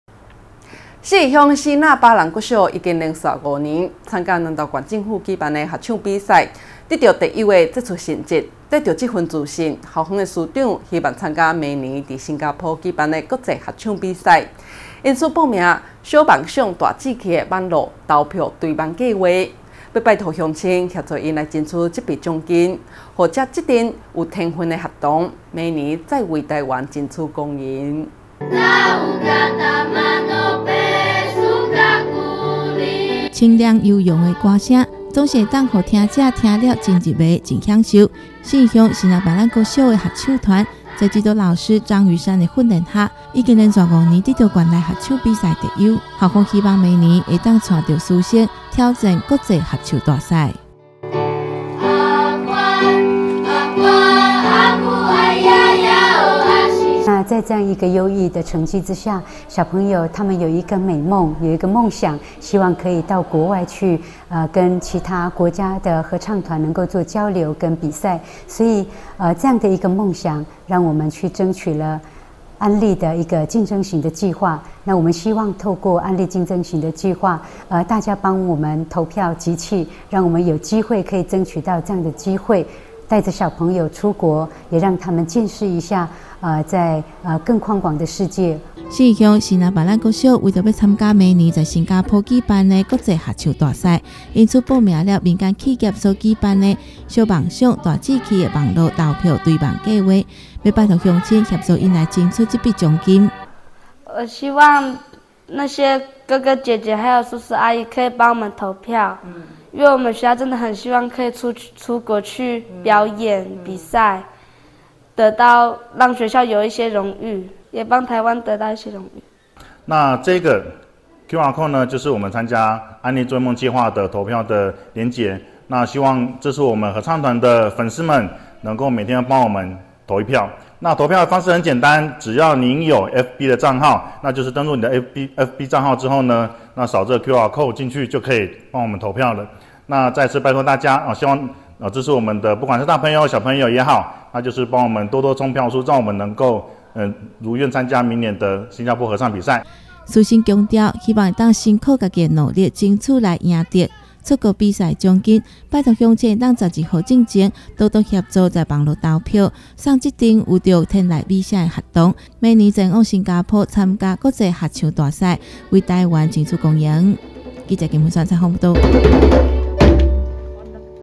信義鄉希娜巴嵐國小合唱團 為圓夢拜託投票 南投新聞
信義鄉希娜巴嵐國小已經連續五年，參加南投縣政府舉辦的合唱比賽得到特優的傑出成績！帶著這份自信，校方師長希望參加明年在新加坡舉辦的國際合唱大賽，因此報名了「小夢想大志氣」的網路投票追夢計畫，要拜託鄉親協助他們來爭取這筆獎金，送這群有著天籟美聲的學童，明年再為台灣爭取光榮。